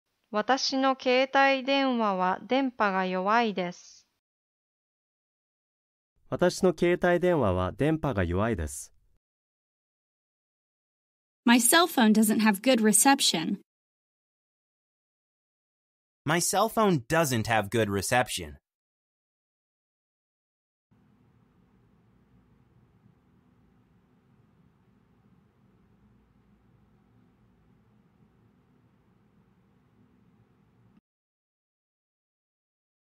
How to say "My cell phone doesn't have good reception." in Japanese
Male Voice:
私の携帯電話は電波が弱いです。

Male Voice Translation:
My cell phone doesn't have good reception.